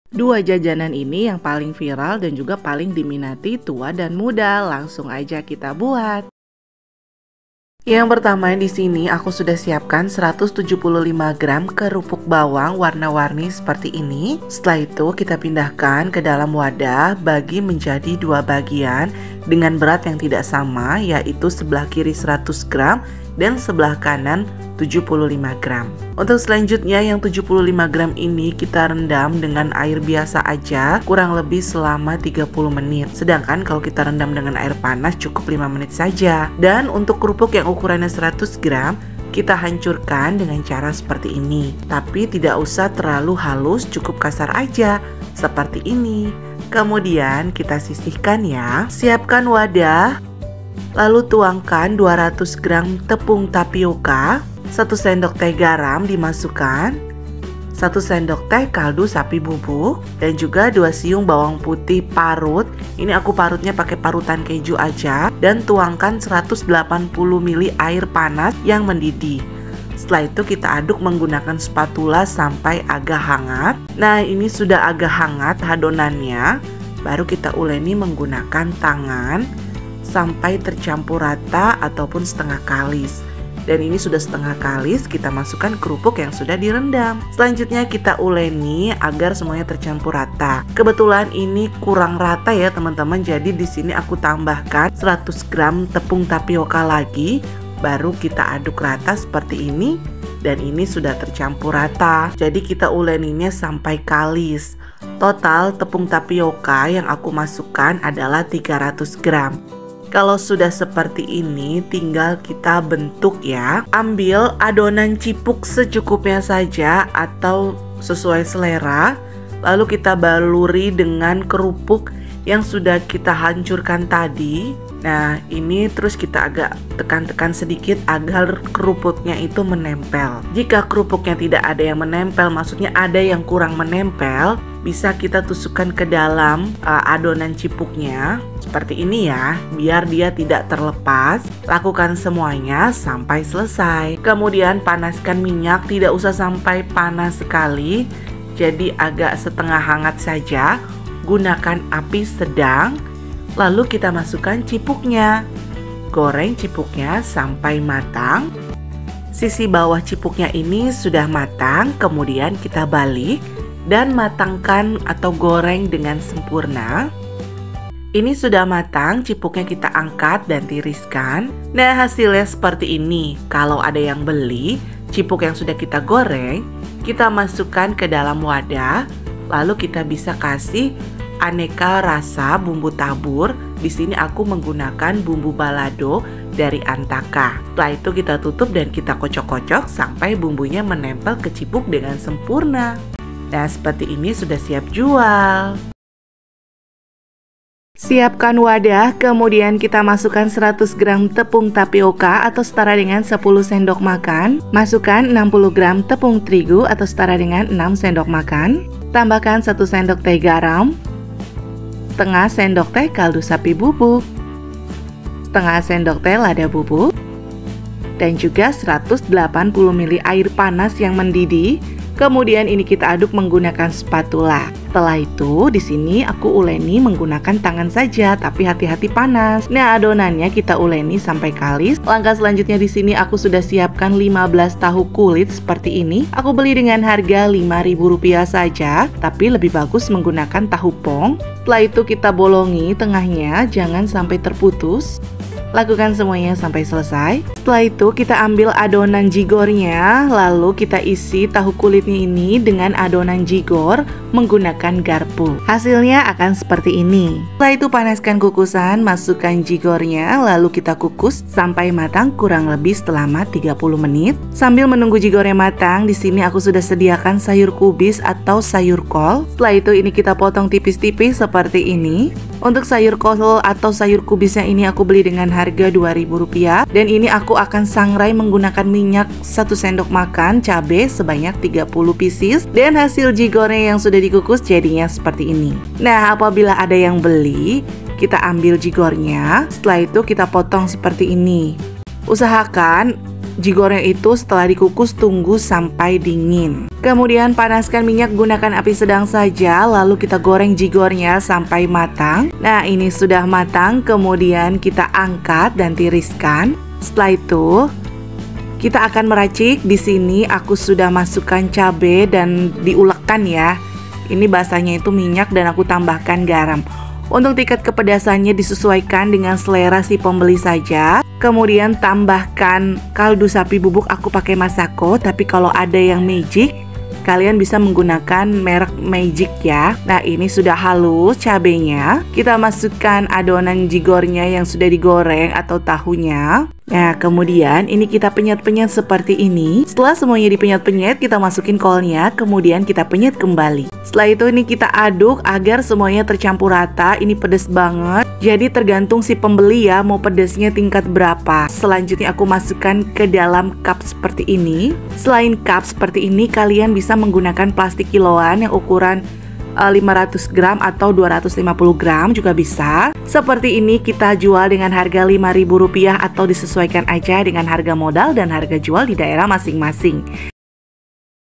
2 JENIS JAJANAN VIRAL YANG BIKIN OMSETMU MELEDAK || SUKSES DI USIA MUDA DENGAN JUALAN MAKANAN INI
CIPUK ":KRAUK ( Aci Kerupuk KRAUK)

bahan
200 gr tepung tapioka+100 gr lagi Rp 2000
175 kerupuk bawang KRAUK ( warna warni) Rp 2000
2 siung bwg putih Rp.500
1 sdt garam Rp.50
1 sdt Masako sapi Rp 100
180 air panas mendidih
bumbu tabur balado

minyak+gas Rp 3000
plastik

hasil 22 pcs(berat masing² 24 gr)

total modal = Rp 7600

saran jual Rp.1000x22pcs= Rp.22.000

keuntungan Rp 22.000-Rp.7600= Rp 14.400

JIGOR ( kanji goreng)
bahan :
100 gr tepung tapioka ( 10 SDM) Rp.1500
60 gr tepung terigu ( 6 SDM) 800
1 sdt garam Rp.50
1/2 sdt Masako sapi Rp.50
1/2 sdt lada bubuk Rp.50
180 ml air panas
15 pcs tahu kulit Rp.5000
30 pcs cabai Rp.2000
Masako Rp.500
garam
harga jual mulai dari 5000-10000

harga jual di sesuaikan daerah masing²